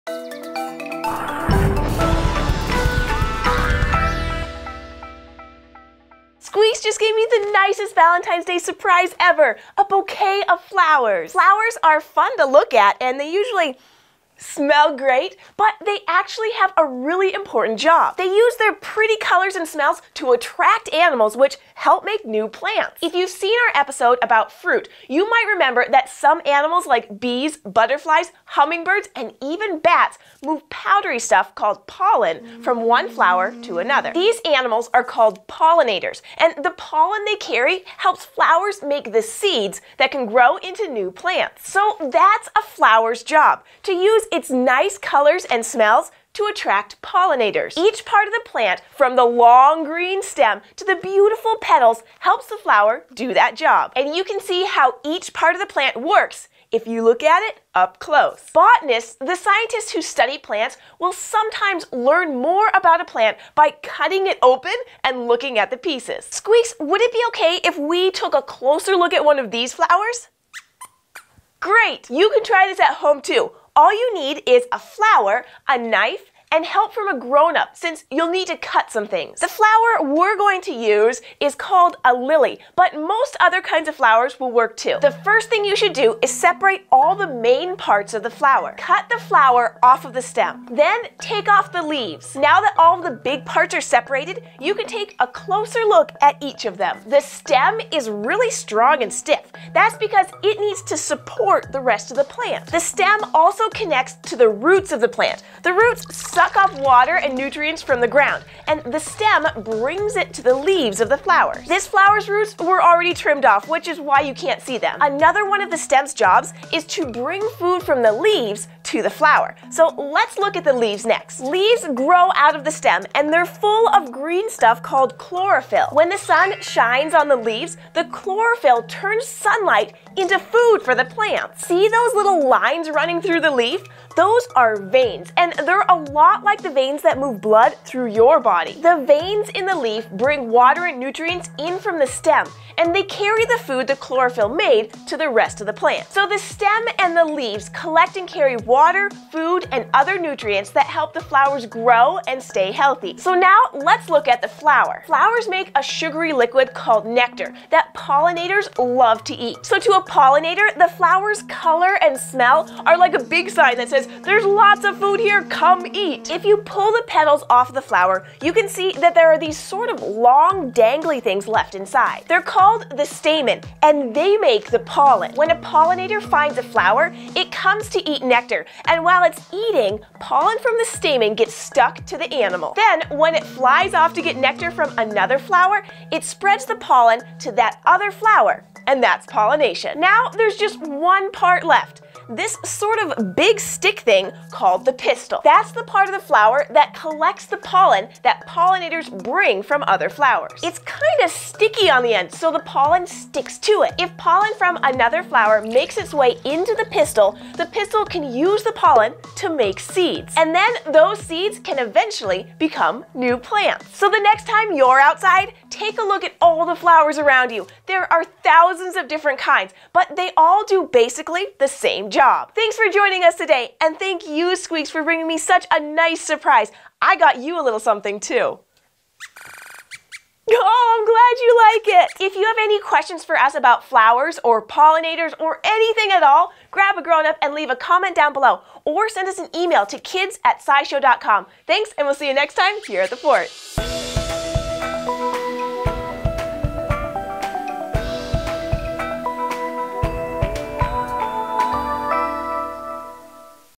Look Inside a Flower! | Science Project for Kids
Squeaks has a Valentine's surprise for Jessi: a bouquet of flowers! Flowers are beautiful and they smell great, but did you know that flowers also do a very important job?